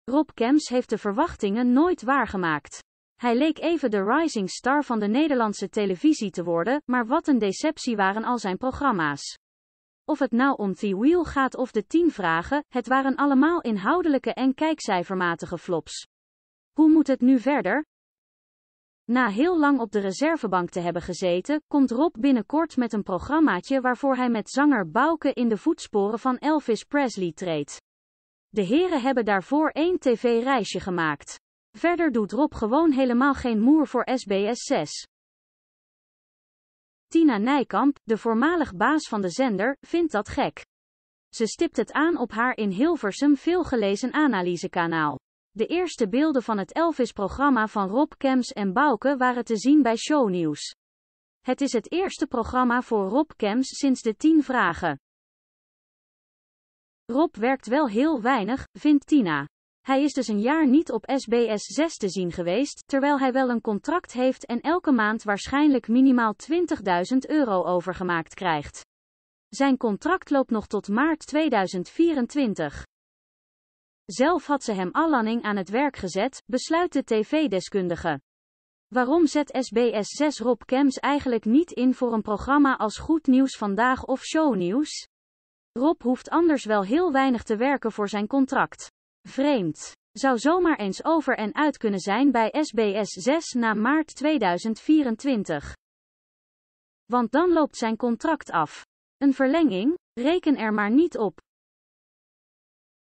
Rob Kemps casht bij SBS 6, maar doet amper wat: ‘Over en uit’
Het lijkt erop dat Rob Kemps richting een roemloze aftocht gaat bij SBS 6. Hij doet amper wat voor de zender, maar casht ondertussen flink. “20 duizend euro per maand!”